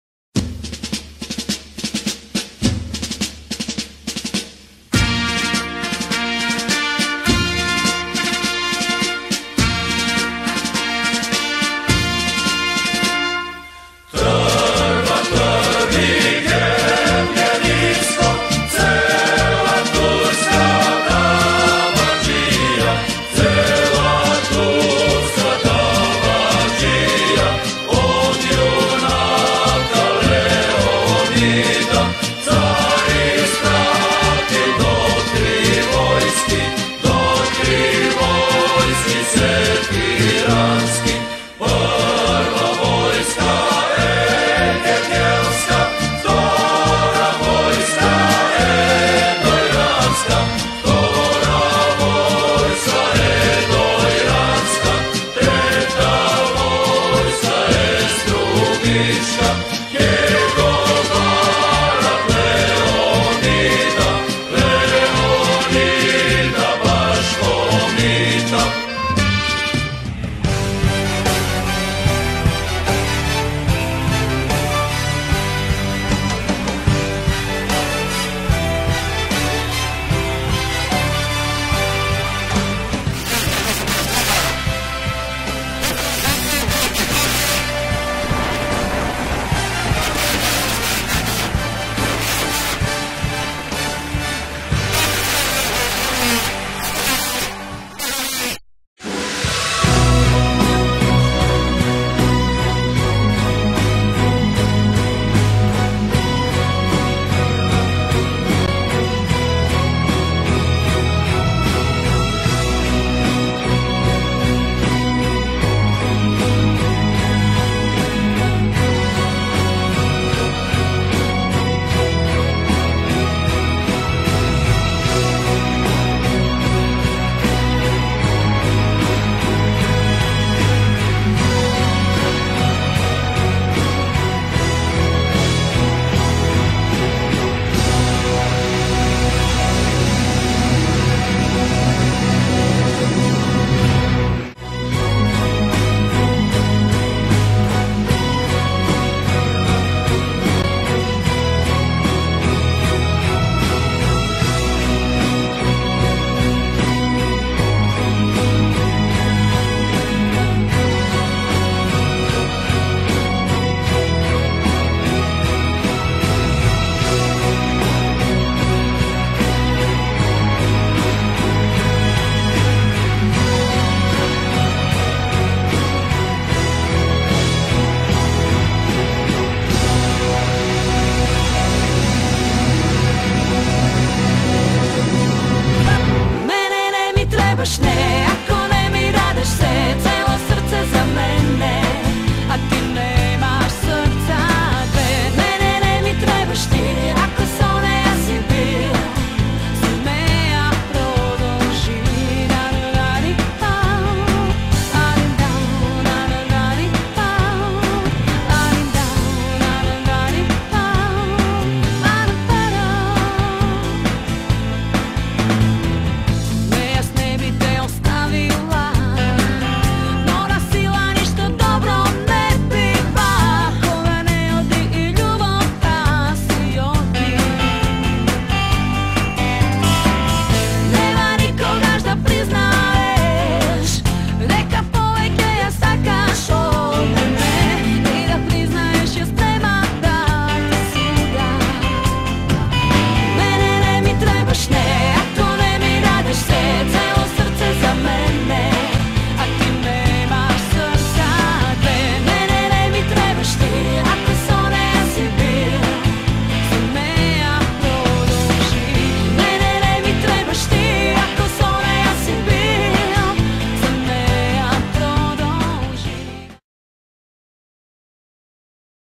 Koledarski Hepening 2015
Ucesnici na Koledarski hepening 2015:
Ideal bend, Teodora, Gligorco, Destrix, Goran i Lence Kukic, Grupa Jupiter, Kate i Neli ti rekov, Vojce Kitanoski, Vlatko Ilievski, Risto Samardziev, Dani Dimitrovska, DJ Tose Todoroff